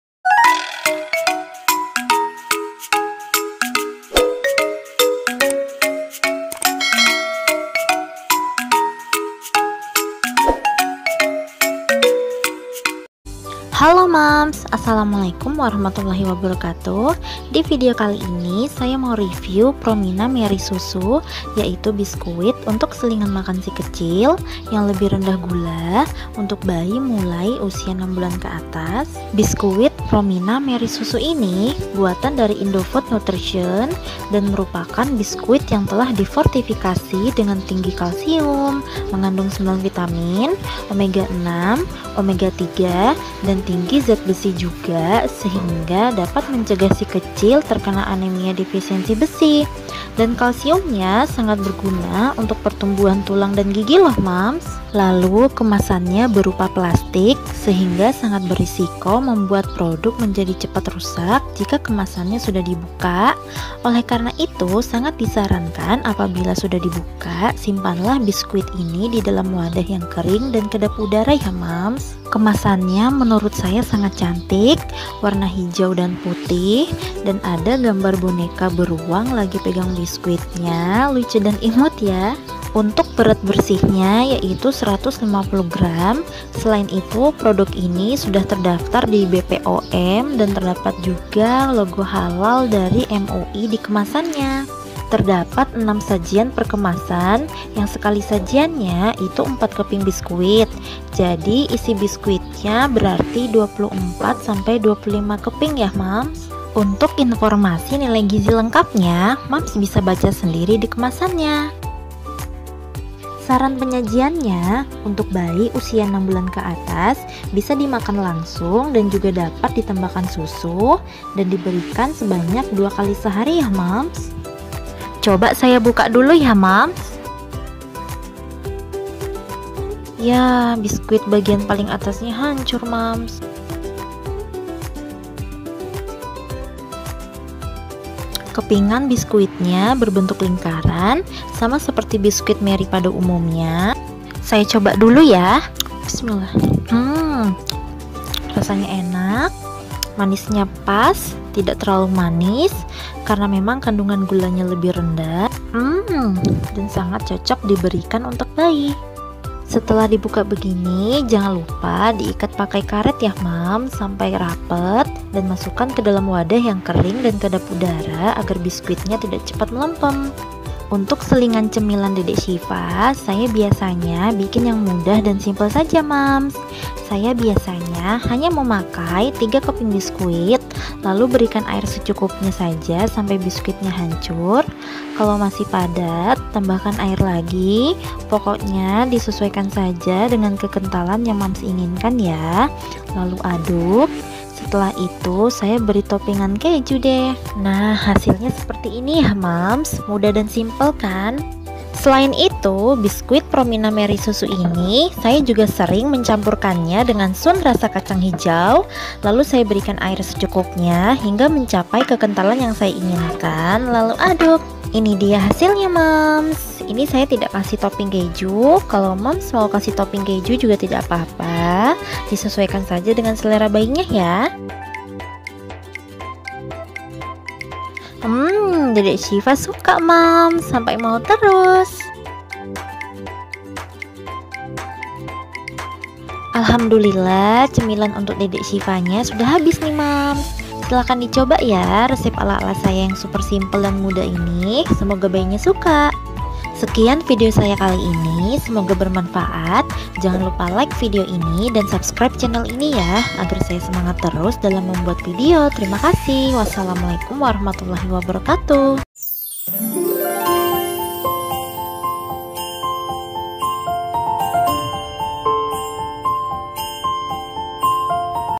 Review Promina Marie Susu Lebih Rendah Gula dan Cara Penyajiannya | Biskuit Bayi🍪| Snack MPASI Bayi
Peralatan MPASI yang saya pakai dalam video ini 👇
Sendok dari Kiddy Baby Food Maker

Kamera: HP Samsung J5 (2016)
Editing video: Kinemaster
Tumbnail: Kinemaster